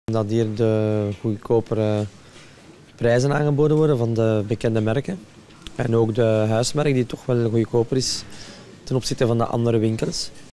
Winkelen bij Colruyt: klanten vertellen waarom
Elke dag opnieuw kiezen klanten voor Colruyt. En ze weten goed waarom.
Ontdek het in dit filmpje.